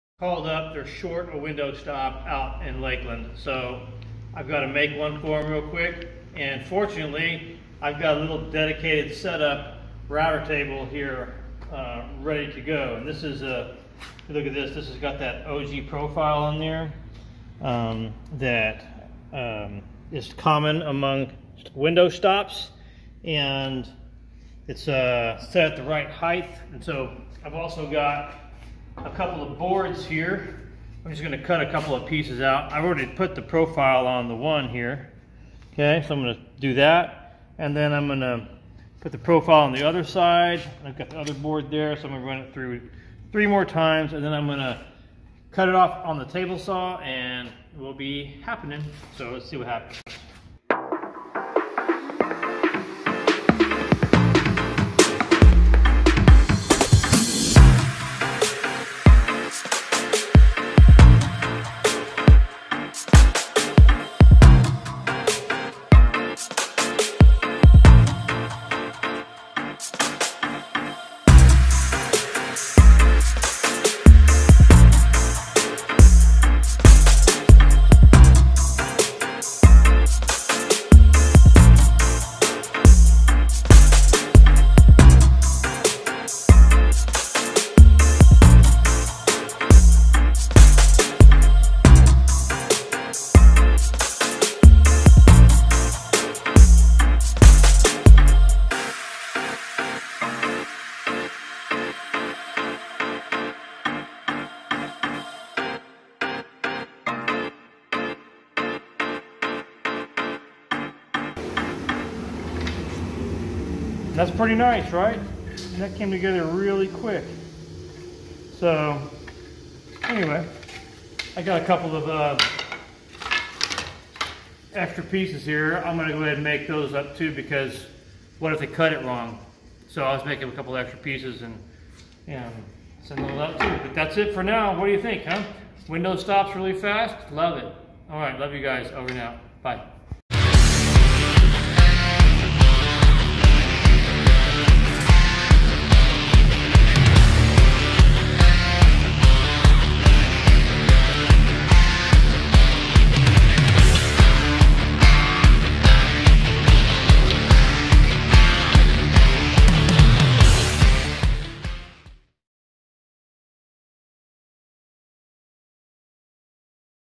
Quick Window Stops
Method to make window stops for a turn of the century double hung window. Utilizes a router table and a table saw. You could also use a handheld router almost as effectively. Hope this helps.

ArtisanArmy.com